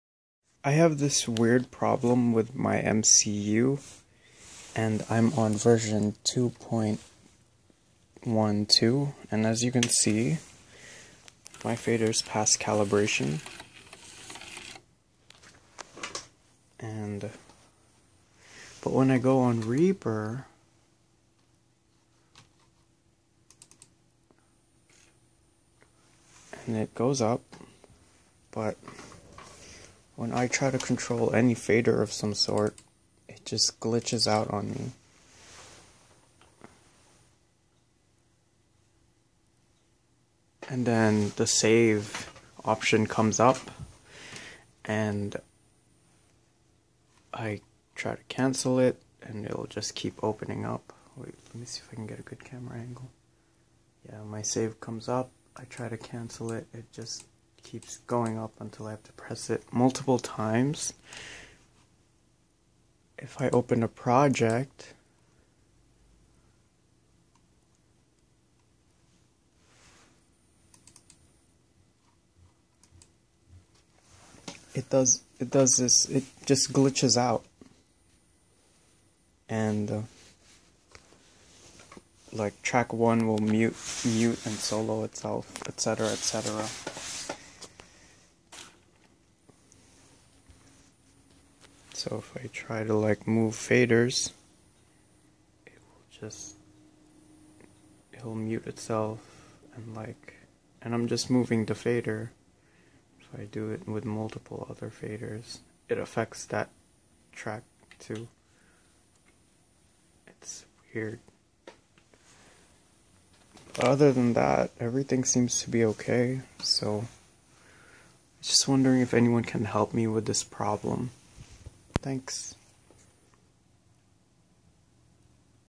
[Fixed!] Mackie Control Universal, First Fader Problem on Reaper
Weird glitchy problem with my first fader on my Mackie Control Universal

I'm using Klinke's MCU pluggin v0.8 for Reaper

My mcu is the older version, the black faced one, but its upgraded to the v2.1.2 mackie control universal.

If anyone can help that would be amazing.

Update:

I got it to work!

I read in other forums out there that their mcu was sending weird midi signals as well, they were using the same midi to usb cable. Like the m-audio uno

So i got myself some midi cables and waited about a week for a midi interface to arrive in the mail. I got a scarlett 2i4, so that i can upgrade from my 2i2, just in case If it still didn't work, at least i didn't burn money.

Long story short. It works, and it was the midi to usb cable to blame

thanks for all your help guys I really appreciate it :]